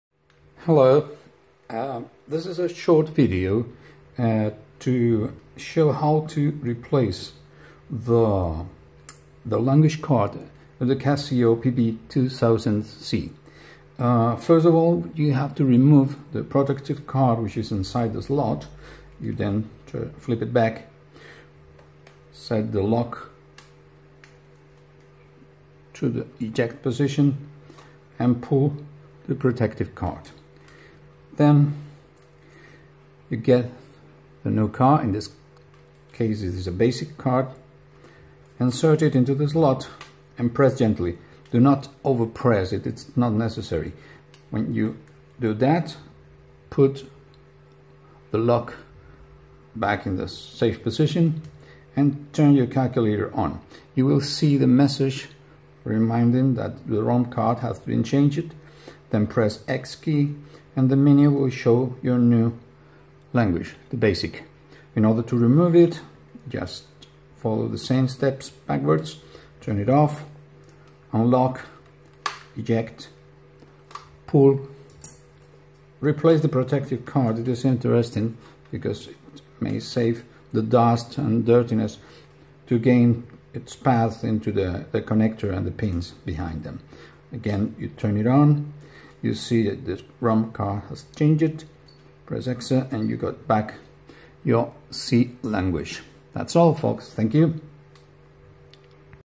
CASIO PB-2000C Language card replacement.
Removing and inserting Language cards on the Pocket Computer CASIO PB-2000C